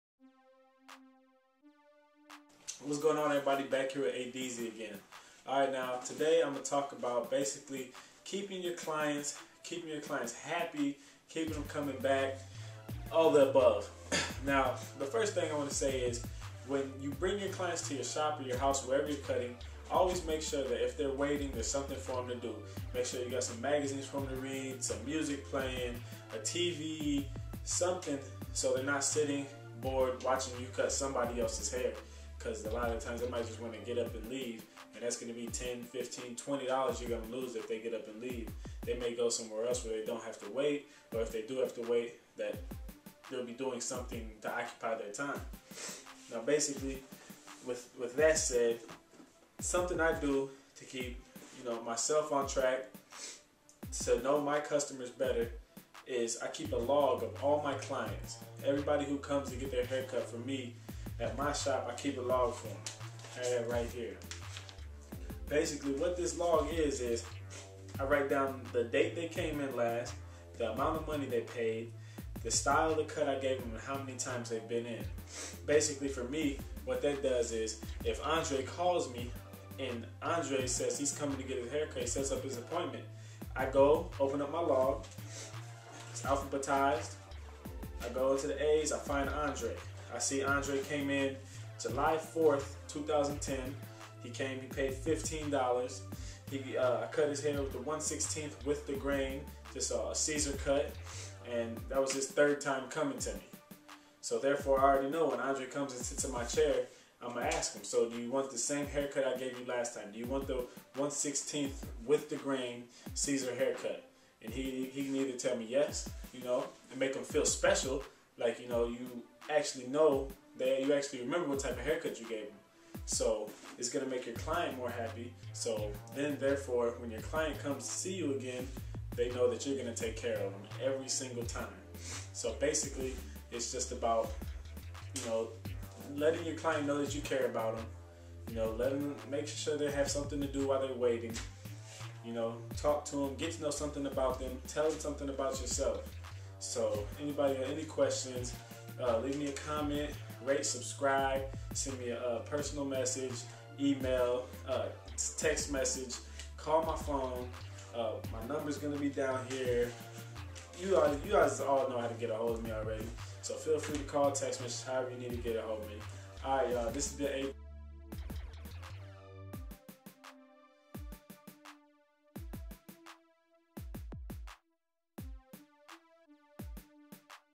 Important Tips For All Barbers and Hairstylist
This video teach's how to tend to your customers and keep them happy and always coming back to you!